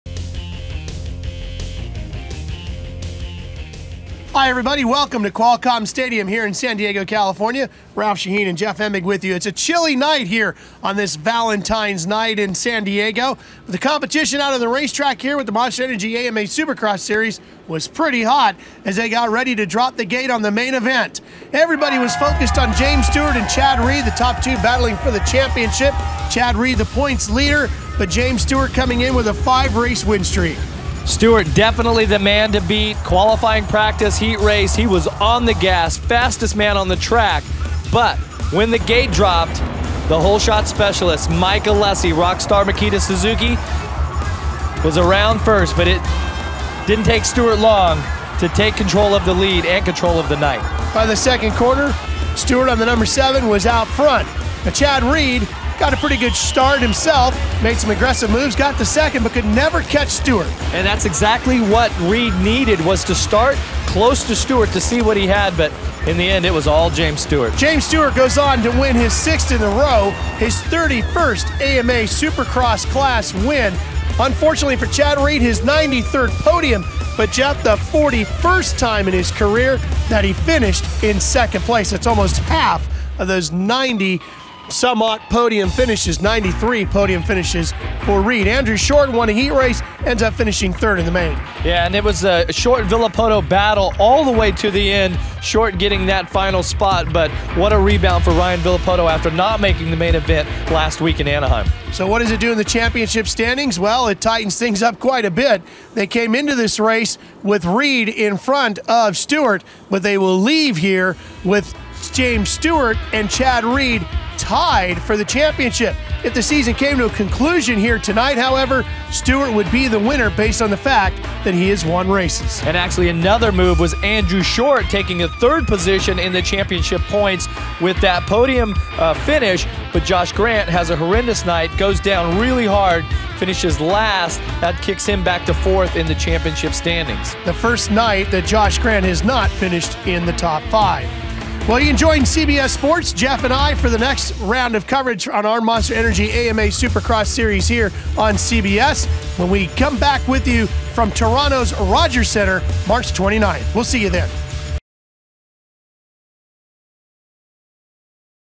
AMA Supercross San Diego
James Stewart won his sixth straight race this past weekend and closed the gap between him and points leader Chad Reed. Ralph Shaheen and Jeff Emig recap all the action from San Diego.